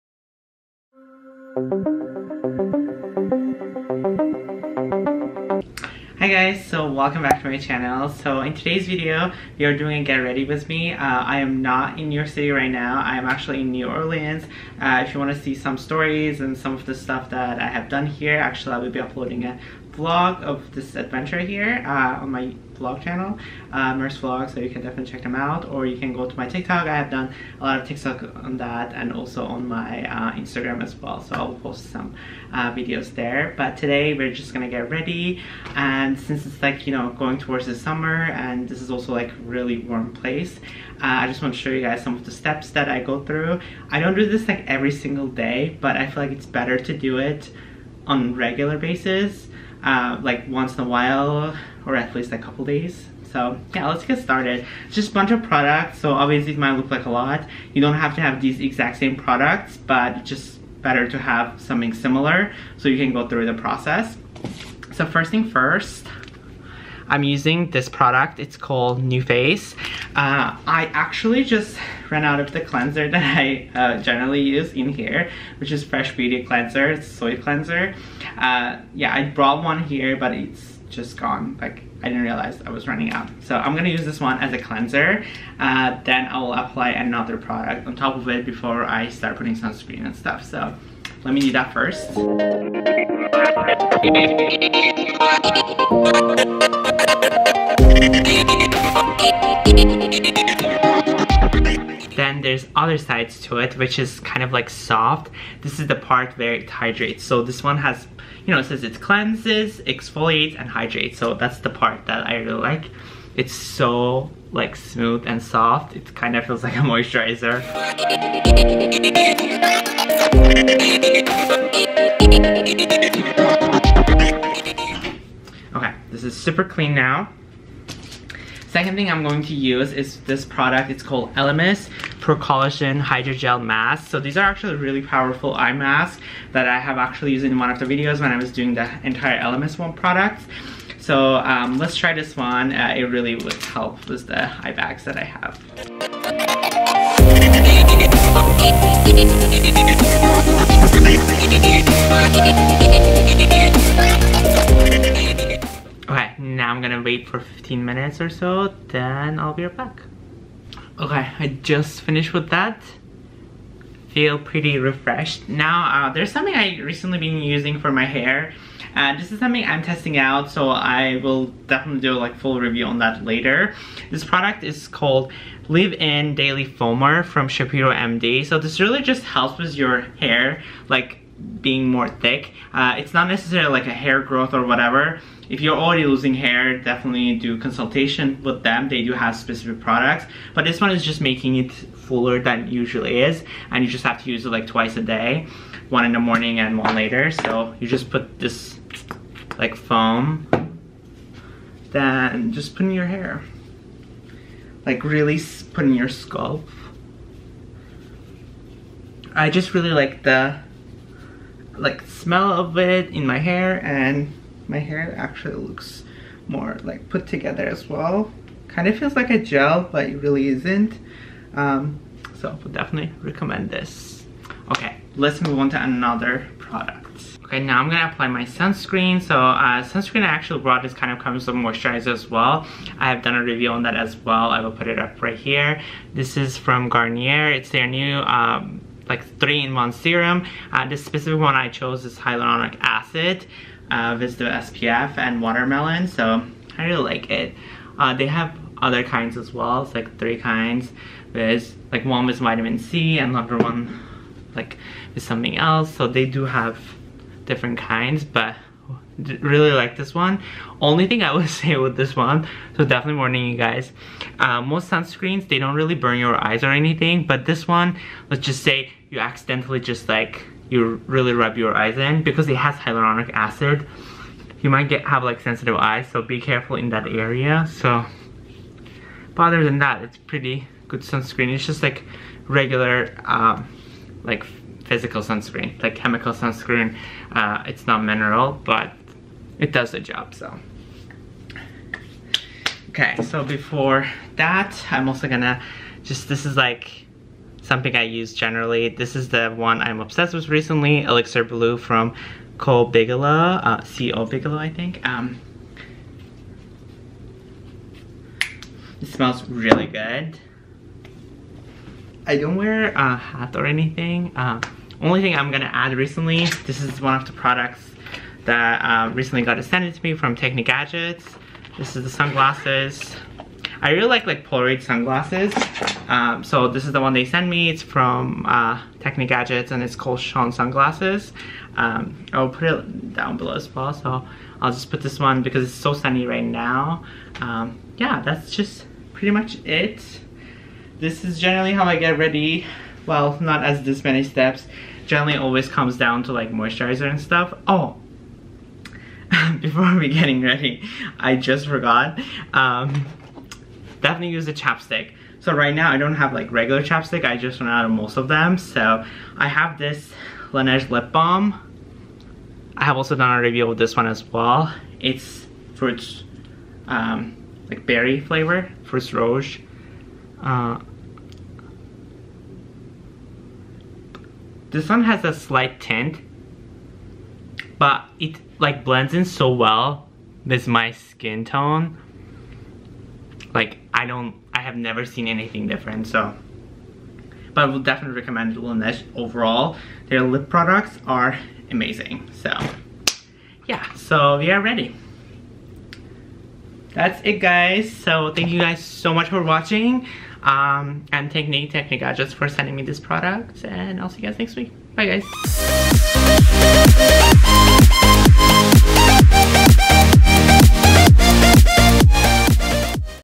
Get Ready With Me To Go Outside In New Orleans I Travel Edition
Get ready with me outside in New Orleans. It's been a while since I did a GRWM so I thought I could show you guys some of the products that I use to get ready when I'm traveling.

Follow Along!